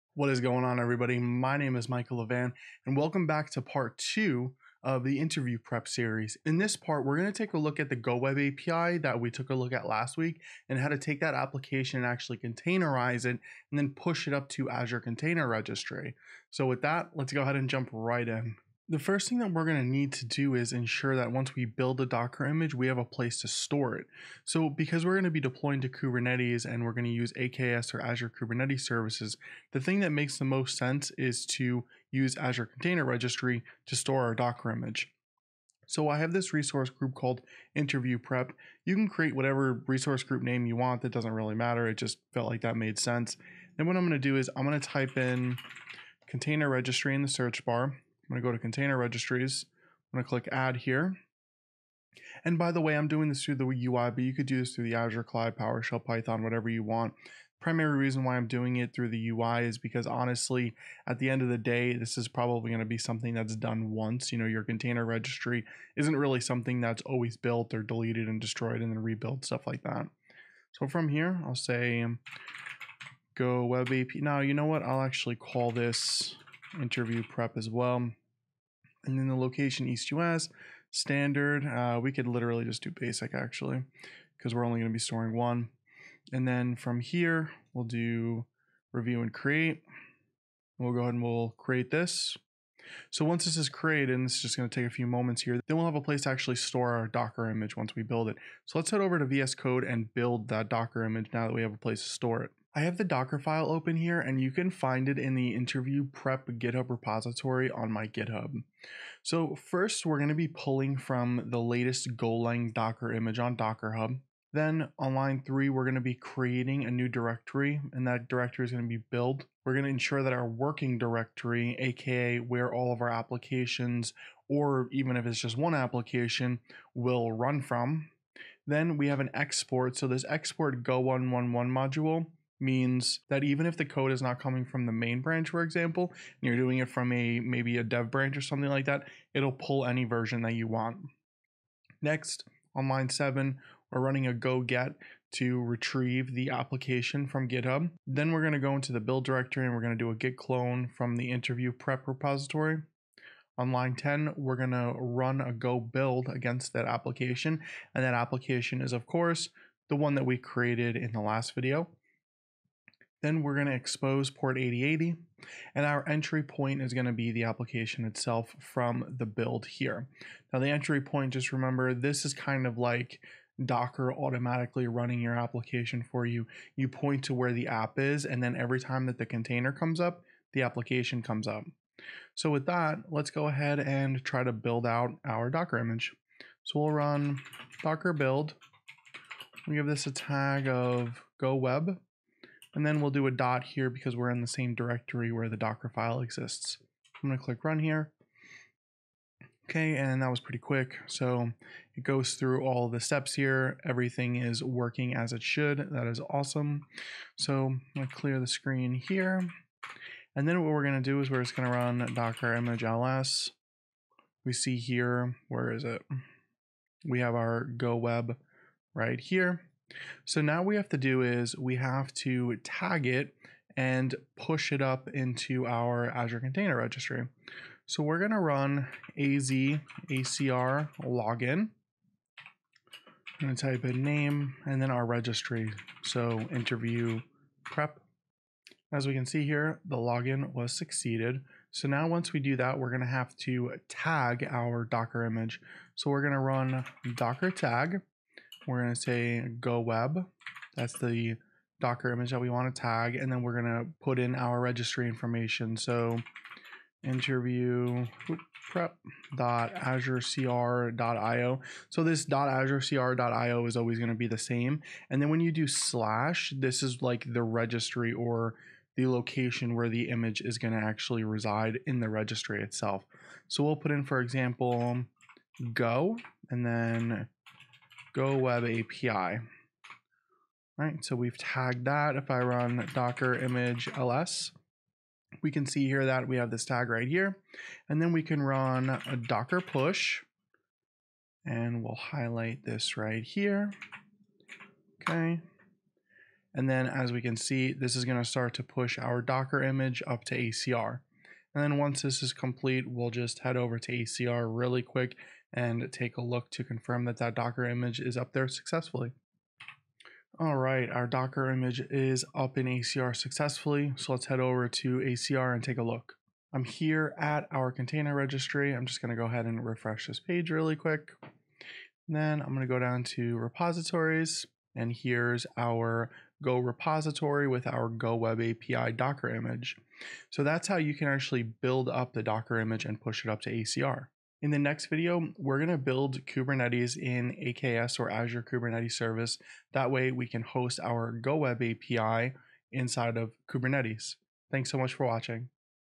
Coding Test For The Cloud Part 2 (Docker)
The OFFICIAL new series for learning the ins and outs of interview prep is here!

For part 2, we'll be over how to take the Go application we built in part 1 and containerize it to be deployed to Kubernetes

This series goes over how to prep if you're going for an interview as a:
1. Cloud Engineer
2. DevOps Engineer
3. Cloud Developer
4. Site Reliability Engineer (SRE)

This series is coming from a REAL coding test that was given to me for a Cloud Native Engineer role.

We'll go over:
1. Creating a web app
2. Containerizing the web app
3. Creating a Kubernetes cluster with code
4. Creating a CICD pipeline with code.

This is a four-part series that will be released weekly!

🔥 Sky Rocket Your Cloud and DevOps Skills Today 🔥